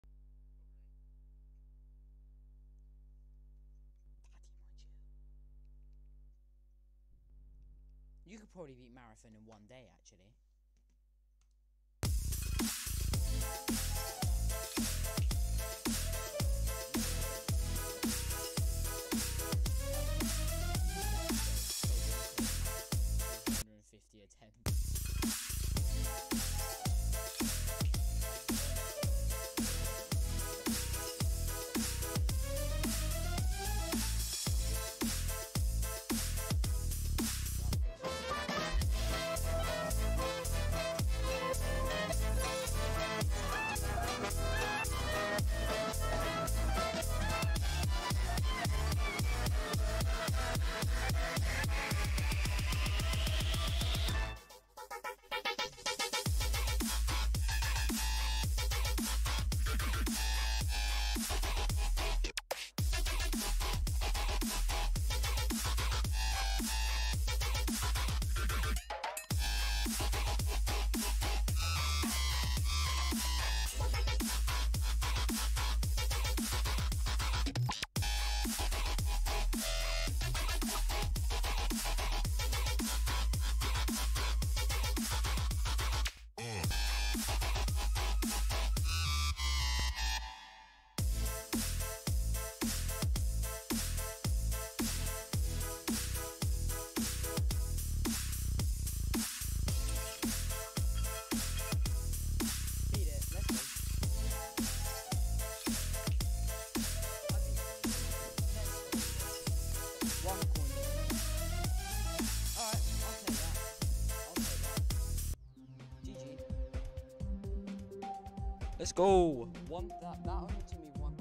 CraZy 100% (Hard Demon) by DavJT
This level is a masterpiece, So fun except the 79% dash orb part.

i totally recommend you play this it is sooooo fun and really not too challenging to get consistent at. and im also planning on beating the whole series so that is something to look out for!!!

attempts:1891

worst deaths: 79%x4, 80%x2, 88%x1